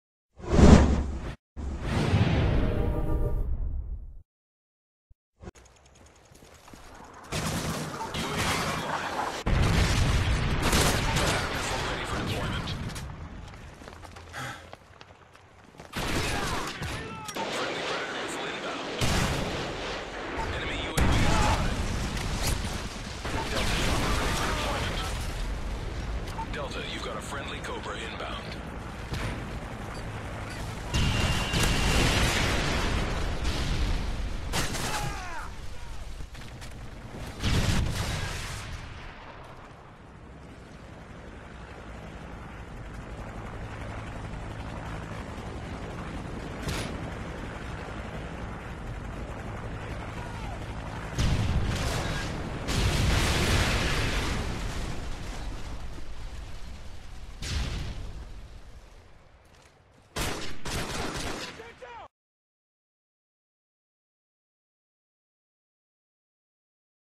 2 Fails for the Price of 1!
A bit longer clip then normal, but we figured change is good! This clip, he started out on a roll, became a bit anxious, and ended up ending his own streak! YUNO LET ME REAPER! :D